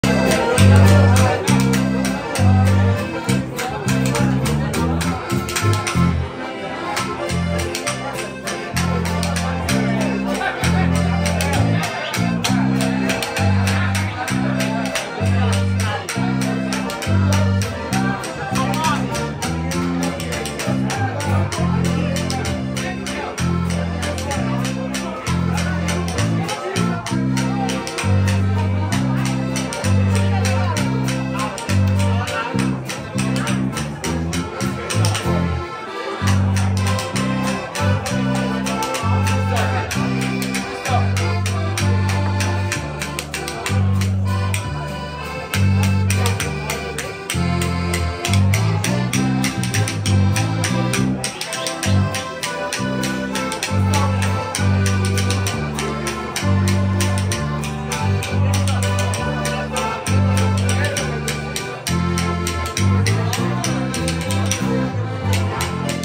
München Bratwurst Glöckl live Musik mit Rudi - Bavarian Music in Munich
Sehr schöne bayerische live Musik im Bratwurst Glöckl - München - Marienplatz im Mai 2023